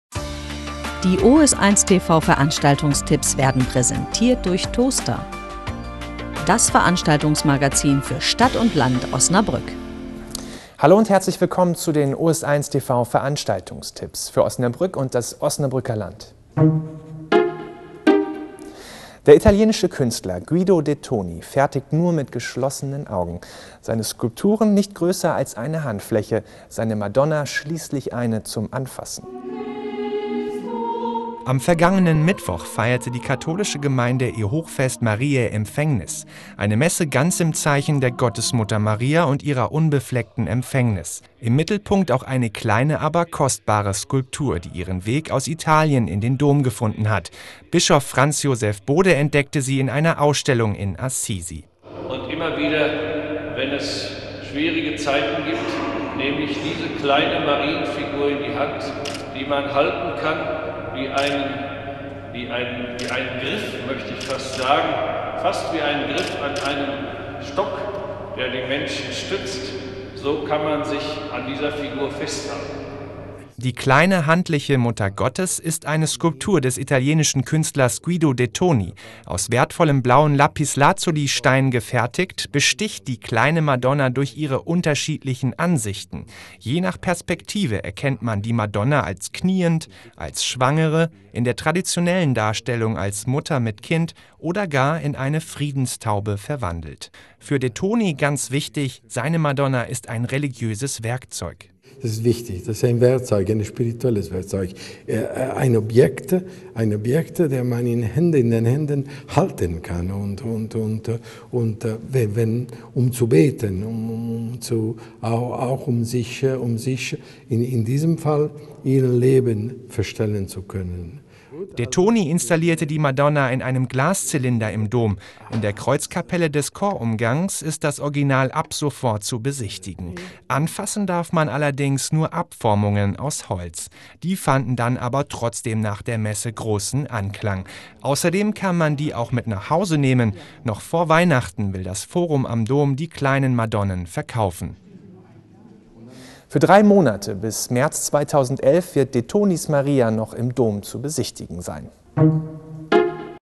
NOZ TV The artwork Maria in Osnabrueck
December 8, 2010 : Inauguration of the work MARIA by Guido Dettoni della Grazia in the Cathedral of Osnabrück